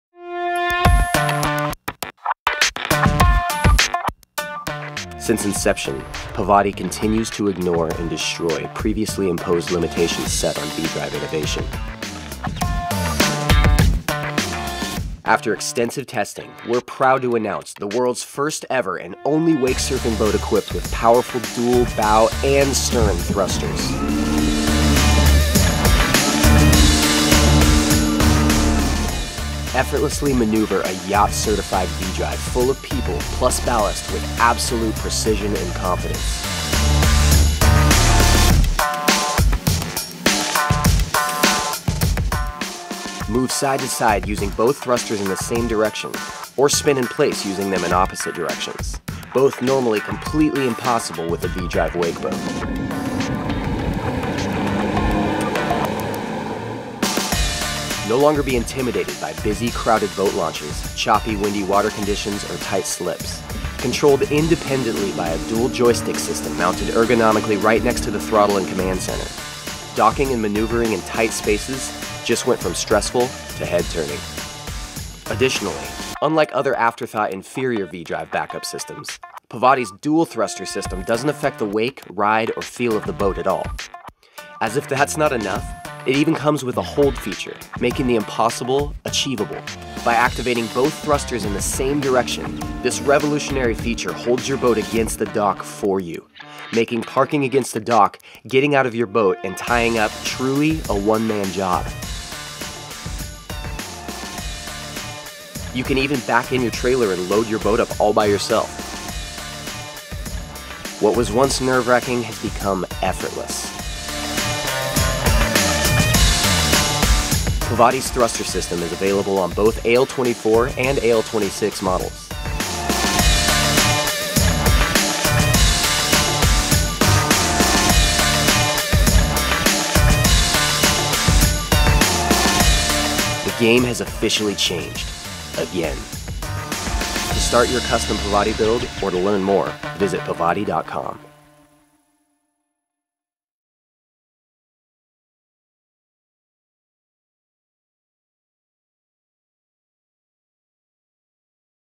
Pavati Wake - Thrusters
The game has officially changed... Again.

Pavati is proud to announce the world's first and only bow AND stern thruster system ever put on a wakesurfing boat. Game over.